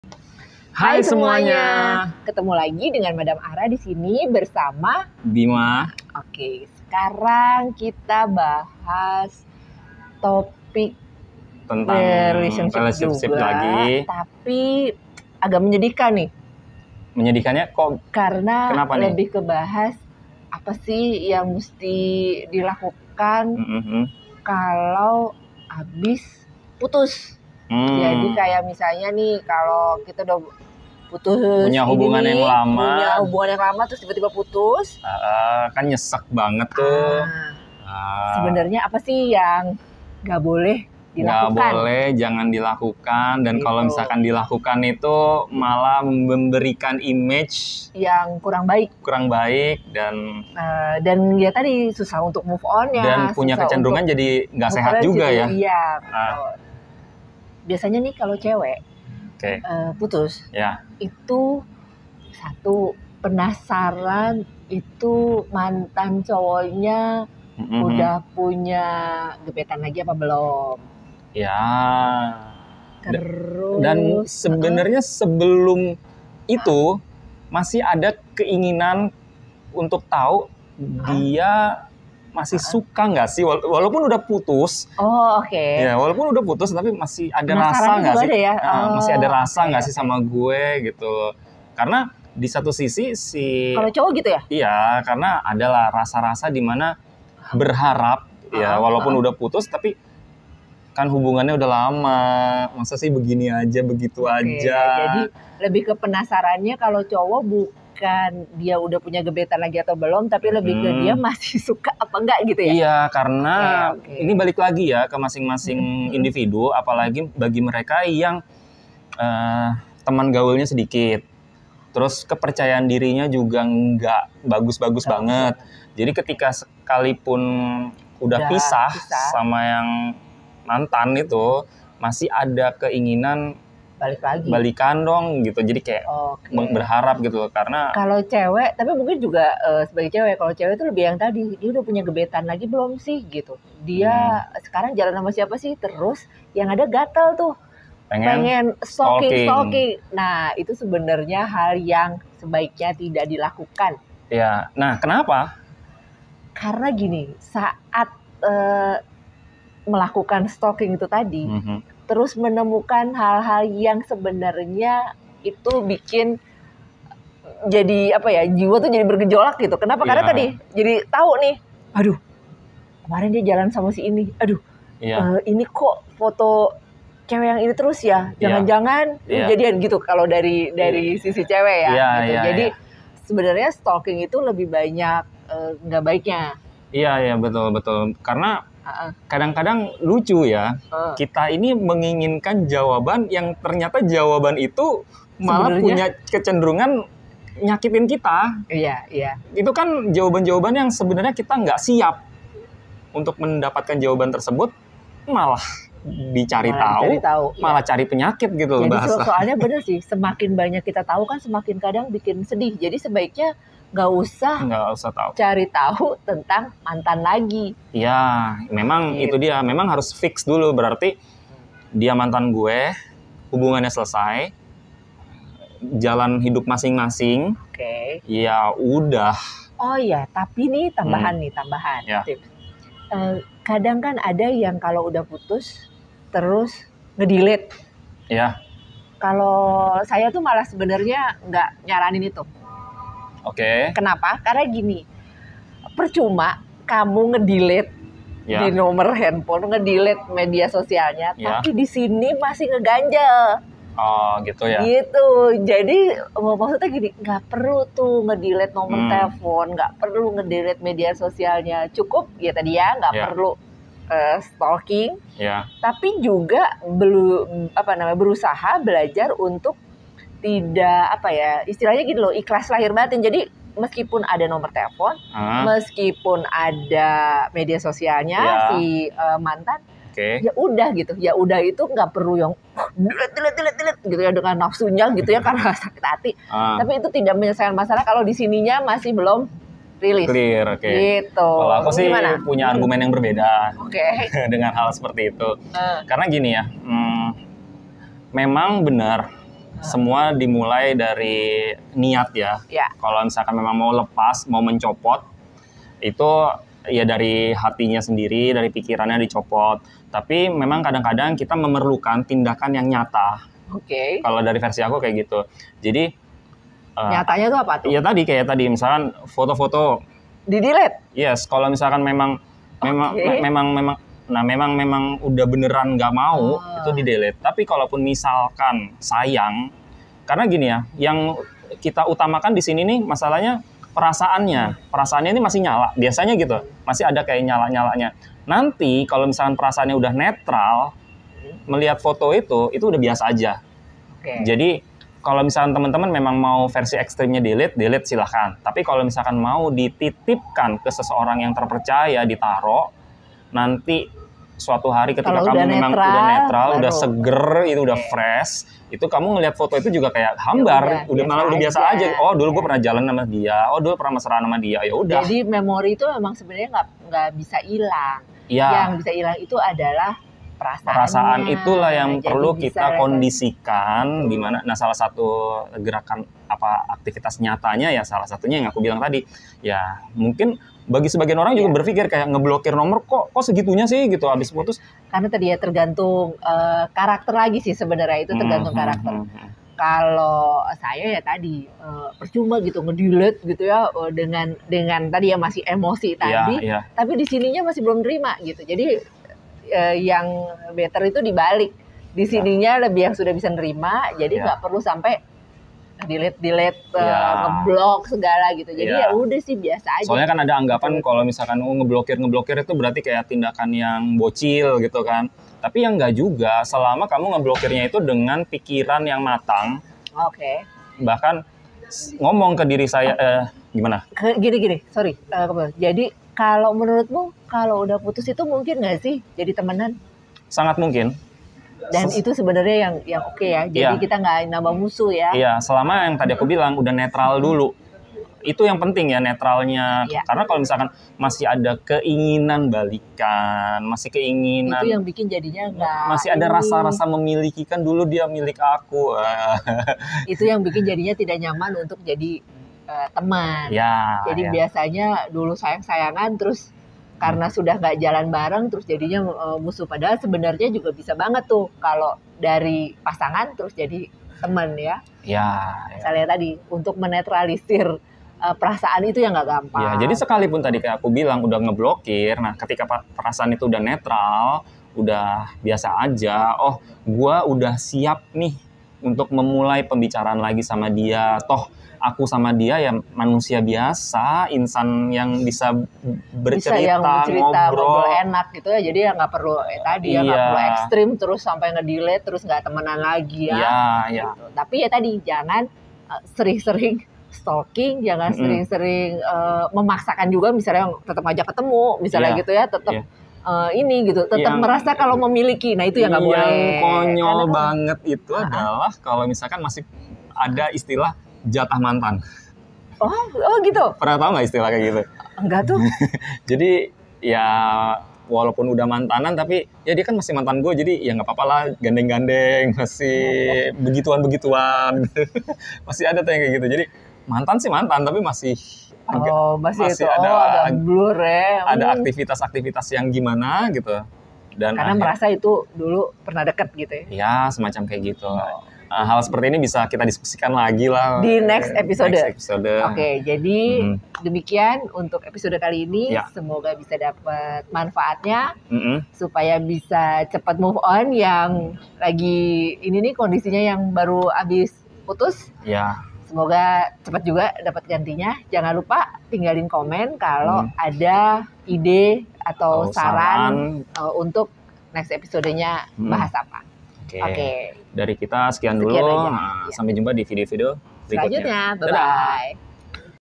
SUPAYA CEPAT MOVE ON, JANGAN LAKUKAN INI KALAU SUDAH PUTUS
Obrolan seputar relationship dibawakan oleh Madam Arra dan Bima...

Tinggalkan pesan di kolom komentar untuk saran/ide tema untuk kami bahas di episode2 selanjutnya ya.. 🙏